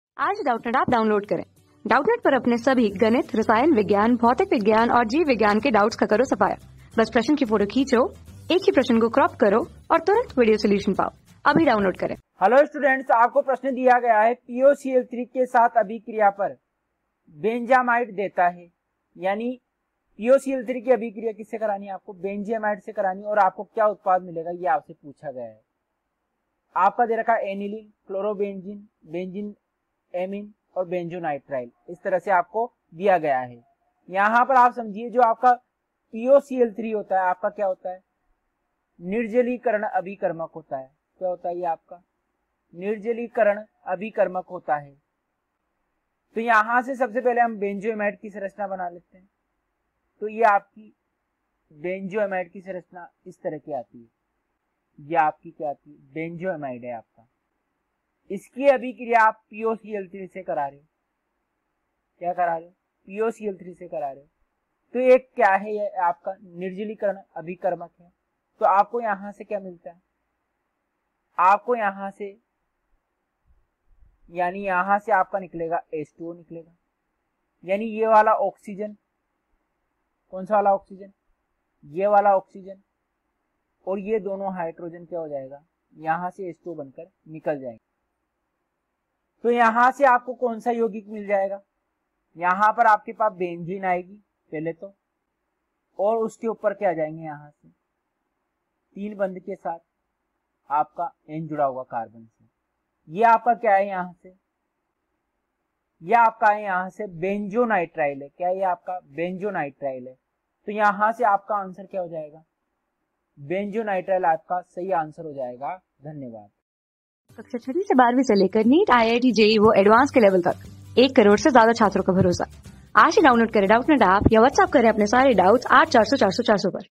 POCl_3के साथ अभिक्रिया पर बेंजामाइड देता है | 12 | NEET TEST 36 | CHEMISTRY | NTA MOCK TESTS
POCl_3के साथ अभिक्रिया पर बेंजामाइड देता है
Class: 12
Subject: CHEMISTRY
Chapter: NEET TEST 36
Board:NEET

👉 Have Any Query? Ask Us.